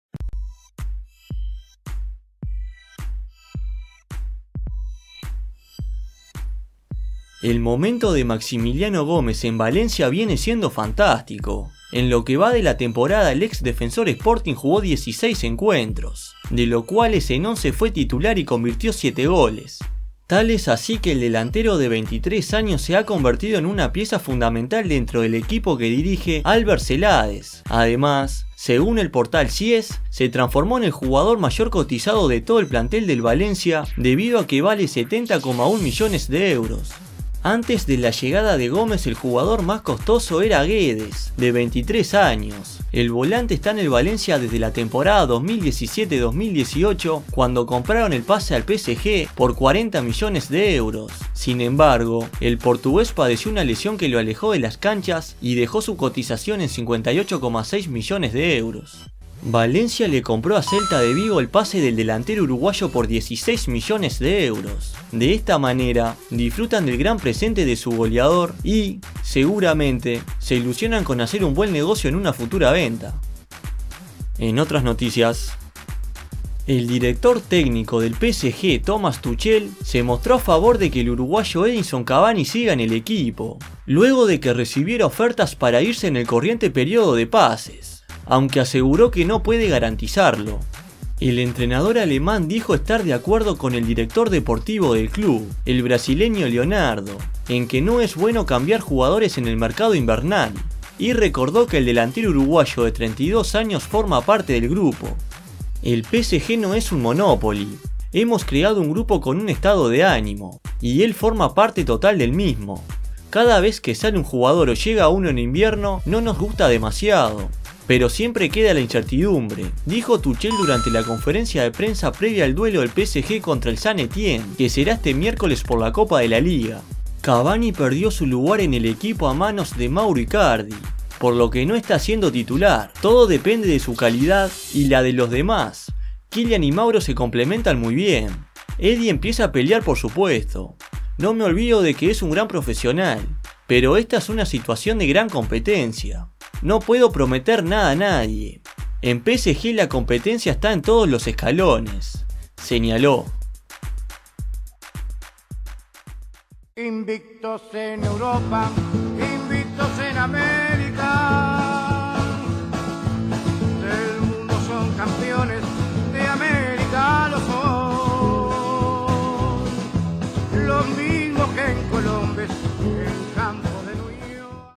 Maxi Gómez el jugador de mayor cotización de Valencia / PSG sobre Cavani: “Depende de su calidad."
En Facebook y en Twitter para salir en el próximo vídeo.

Fútbol UY

Comenta en la caja de comentarios de que quieres que sea el próximo vídeo.